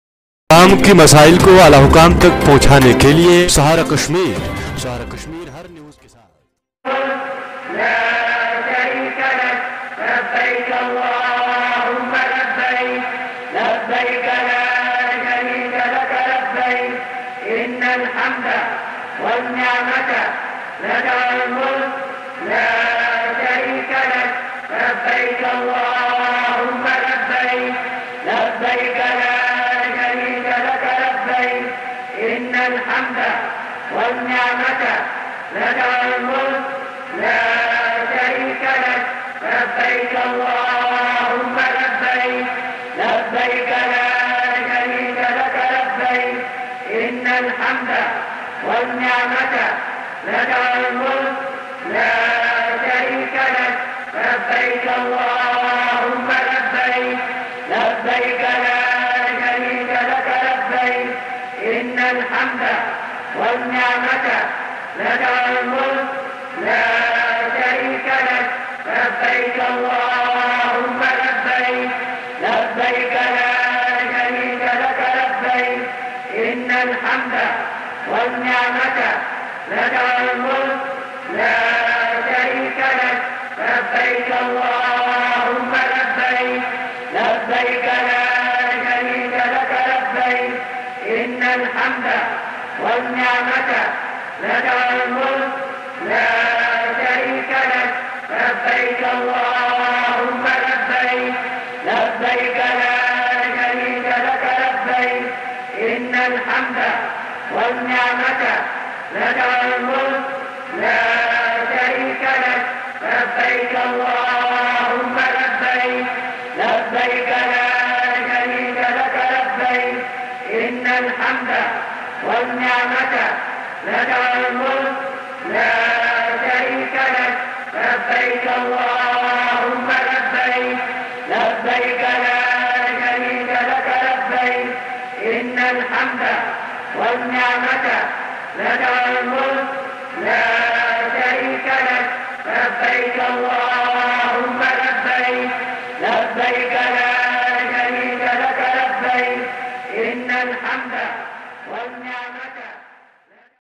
They flock to the holy shrine of Arafat
Filled with an atmosphere of faith filled with reverence and serenity.